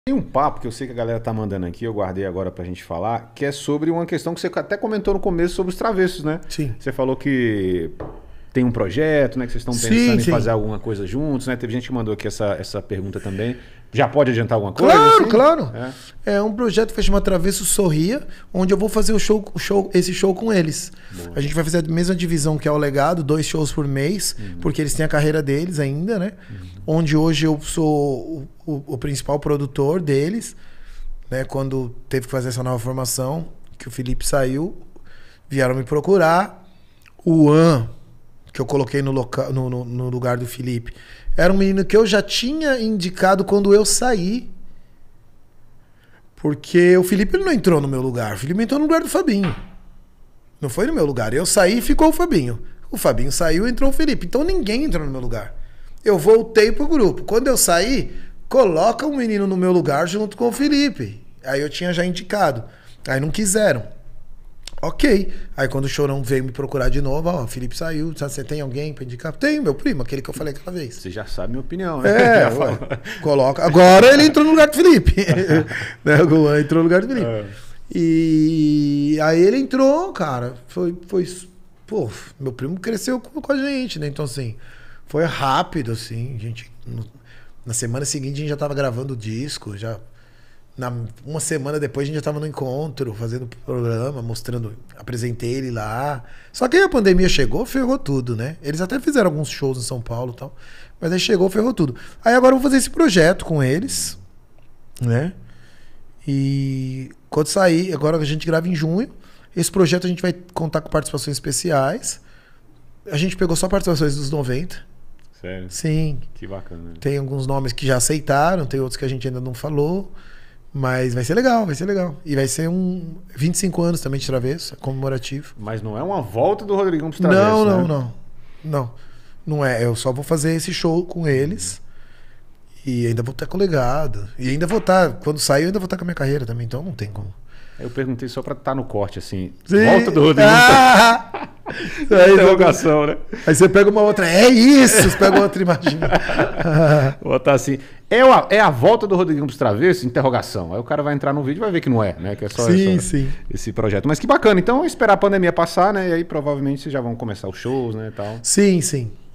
RODRIGUINHO SOBRE O NOVO PROJETO NA SUA CARREIRA | Brito podcast
Um super papo com o cantor Rodriguinho. Neste vídeo ele conta sobre o novo projeto com os Travessos.

ANFITRIÃO:
Leandro Brito - Twitter @britoleandro